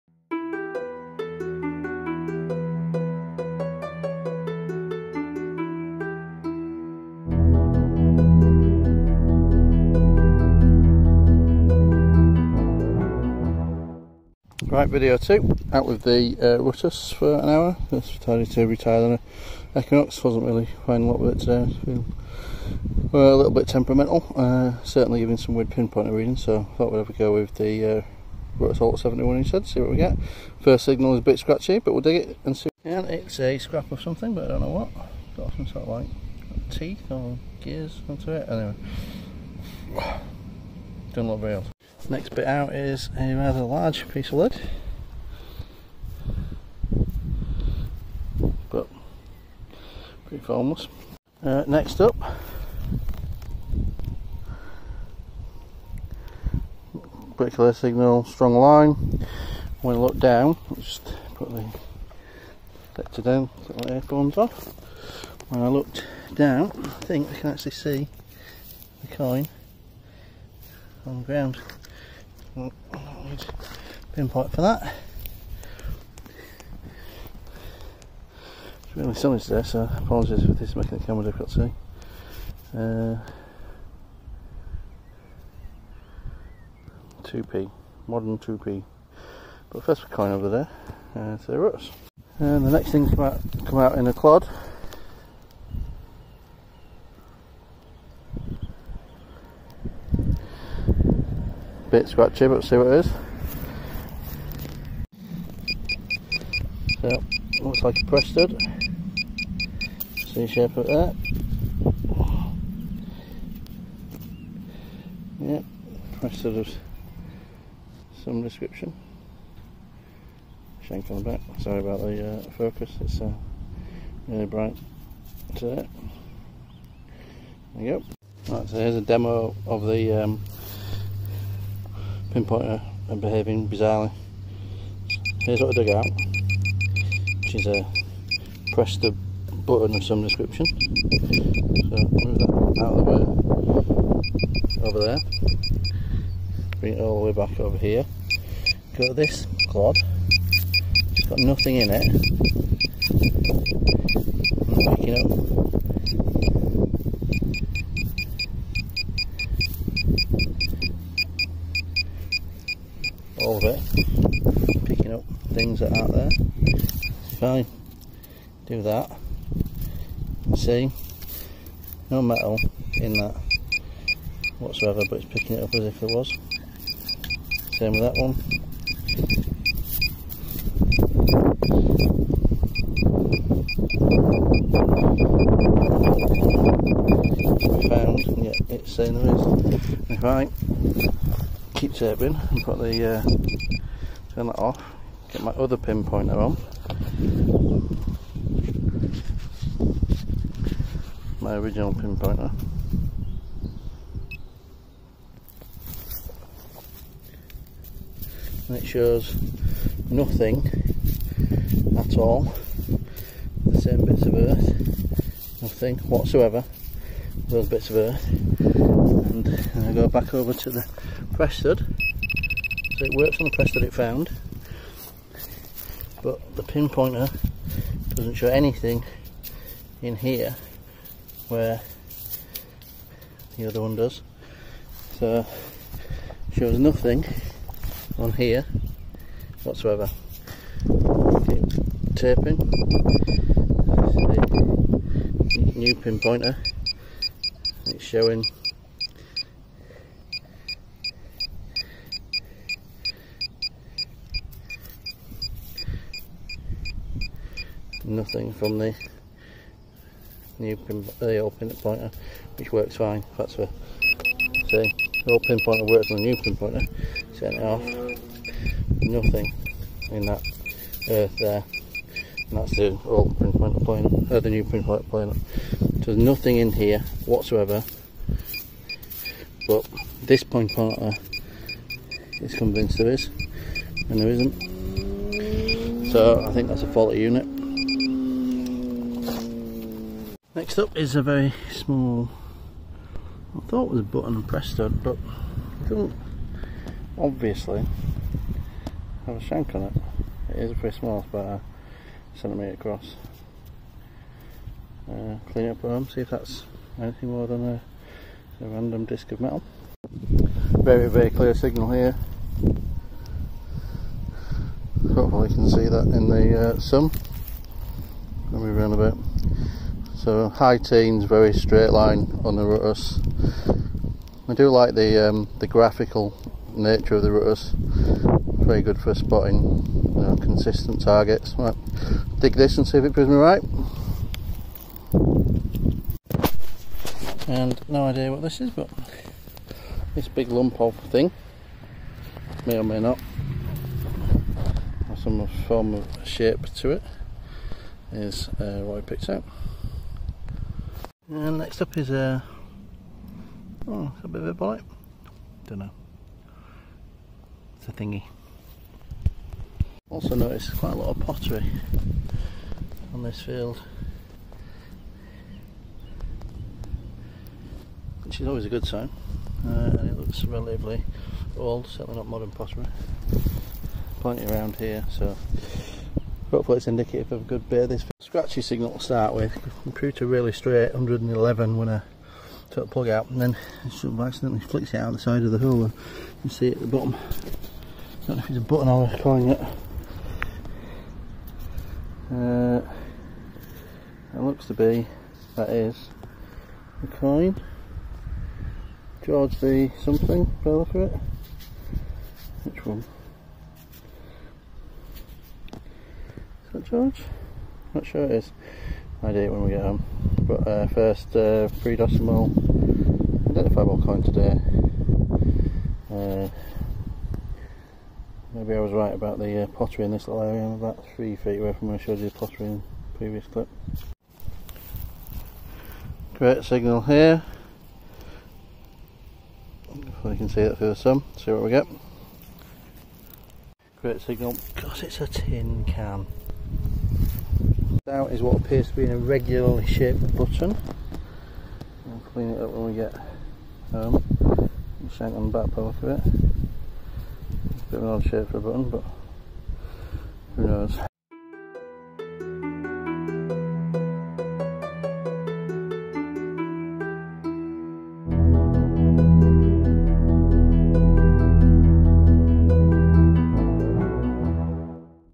Rutus Alter 71, short hunt and a couple of coins
A quick hour with the RA71, with a look at some weird behaviour from my new Ebay pinpointer... have asked for a refund or replacement!